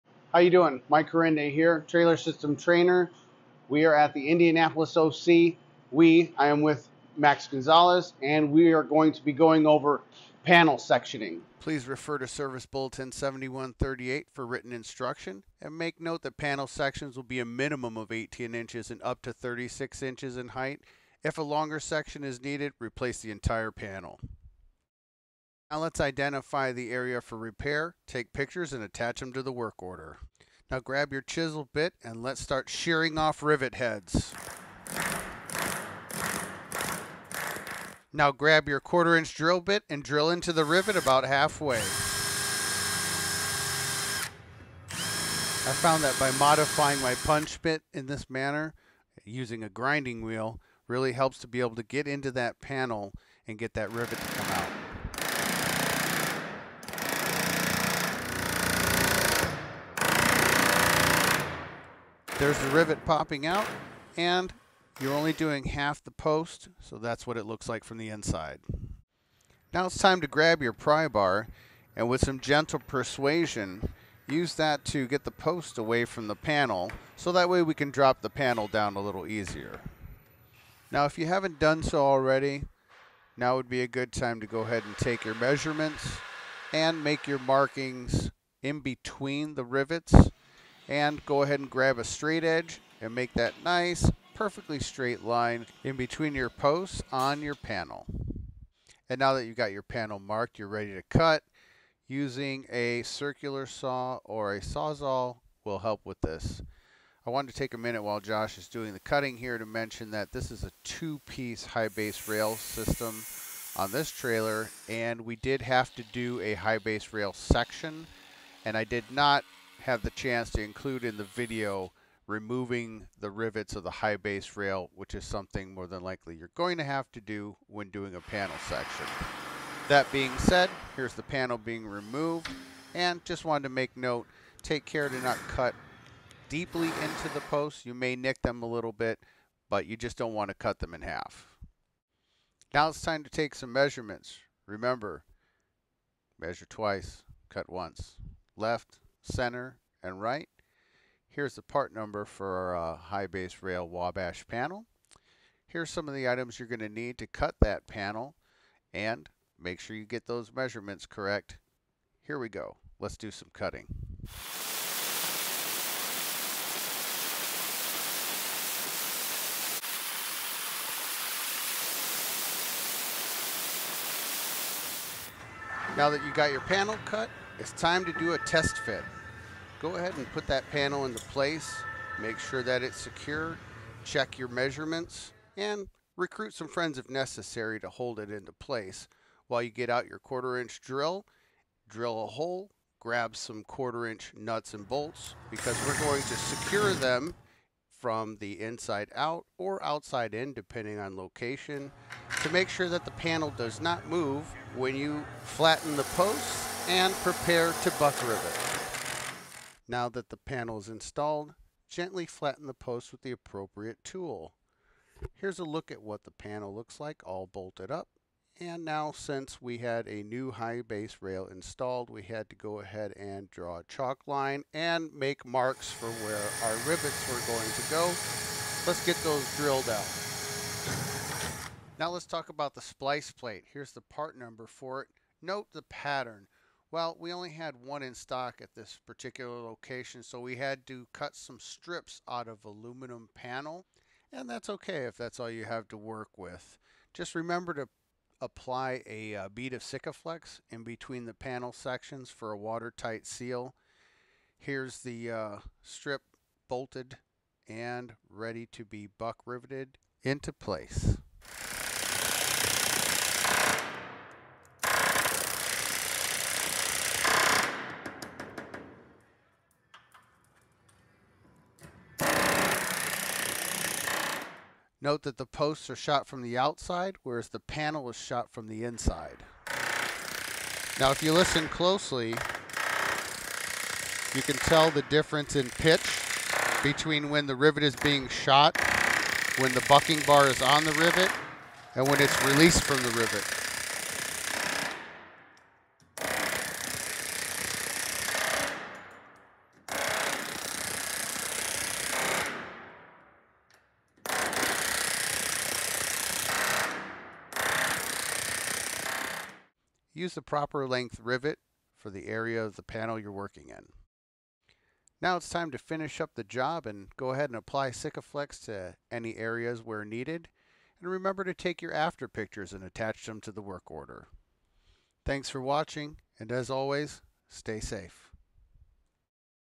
Van Trailer Composite Panel Section
Please refer to SB 71-38 for written information. This is meant to help guide you through a composite panel section. Different trailer brands have different high base rails, make adjustments accordingly.